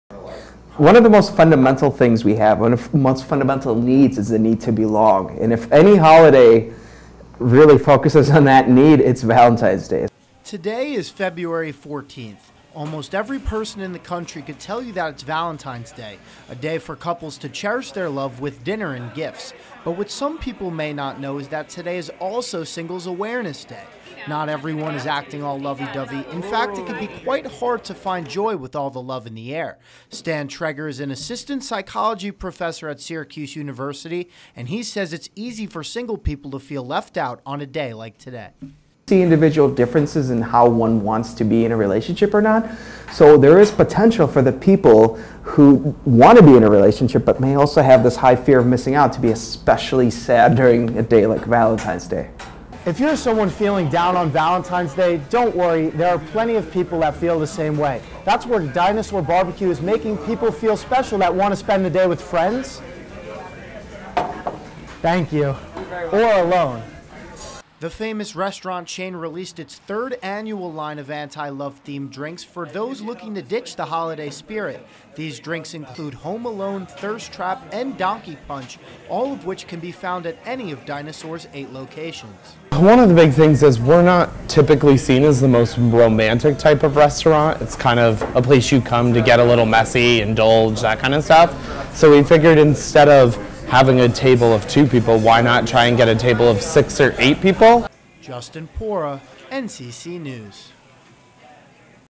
Singles Awareness Day
Valentine's Day could be one full of romance and joy, or one of sadness and major F.O.M.O. NCC News' Justin Paura reports on one Syracuse restaurant wanting to make those out of relationships feel welcome today.